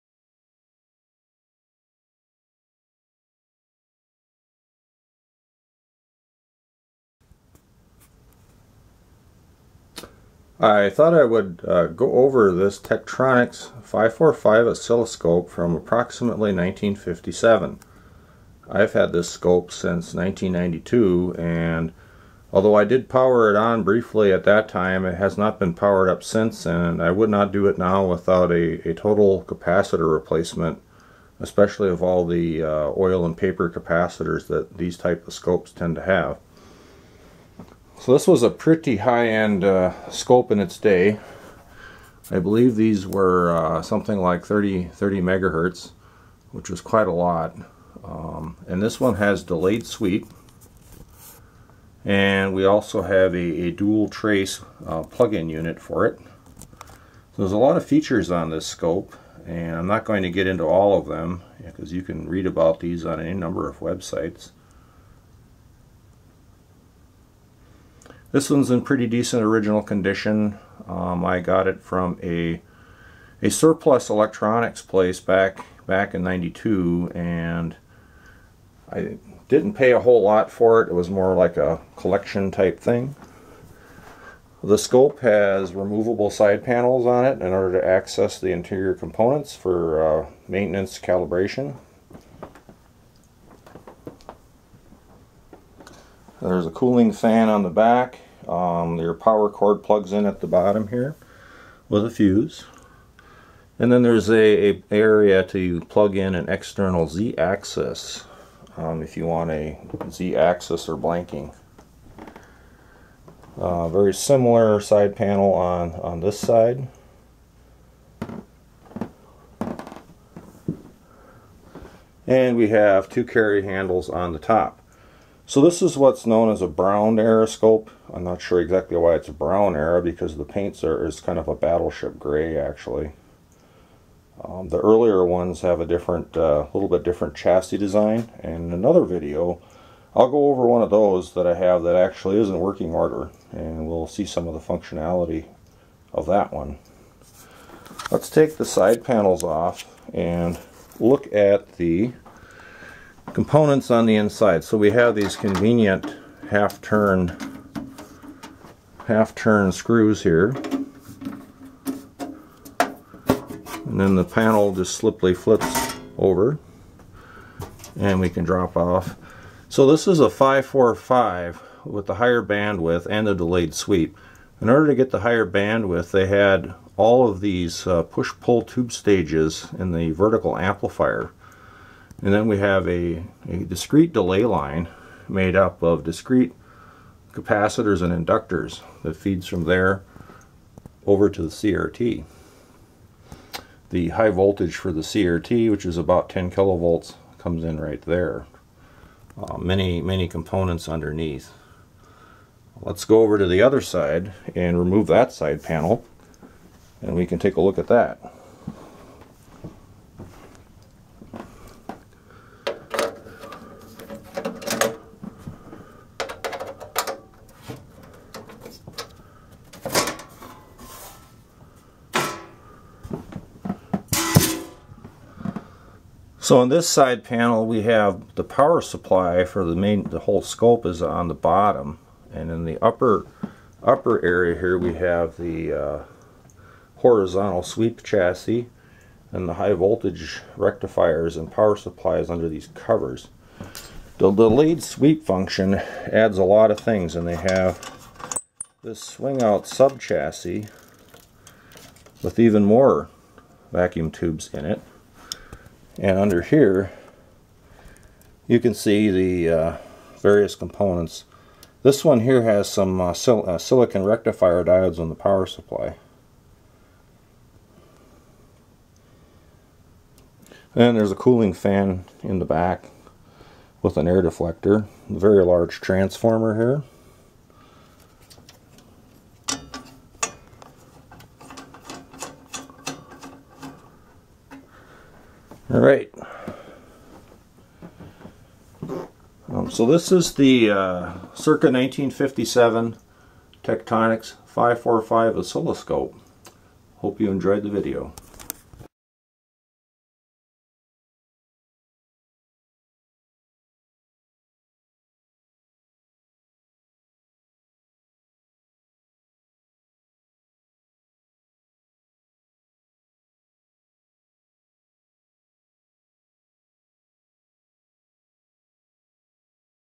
Tektronix 545 Oscilloscope
Overview of a vintage 1957 Tektronix Type 545 Oscilloscope.

I've had this 'scope since 1992 and it was "sorta" working at that time.  Needs a complete going through of the capacitors now.  Its completely original inside.  My experience with another slightly older scope of this series is those "bumble-bee" and "black beauty" capacitors need to go or it will be a nightmare of constant issues of failures and flaky operation.

For a few years around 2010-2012 I had it in my office cube at work for display and had the info sheet seen at the end of the video.  Inflation adjusted prices are now out of date!